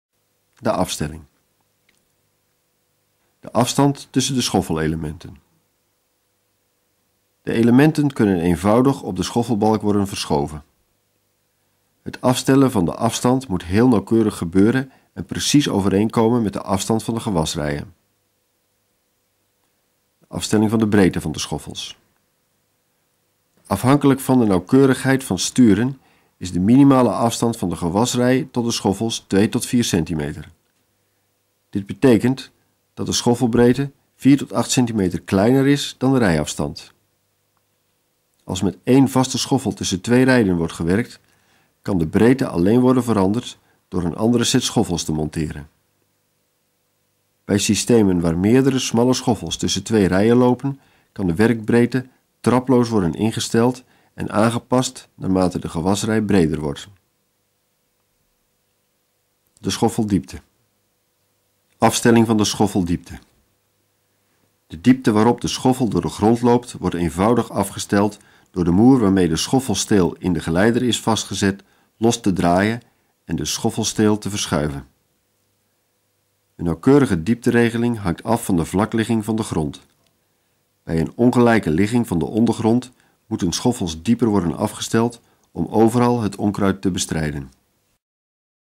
Afstellen van de schoffelbalk
Hoe stel je de schoffelbalk goed af om optimaal onkruid te kunnen wieden.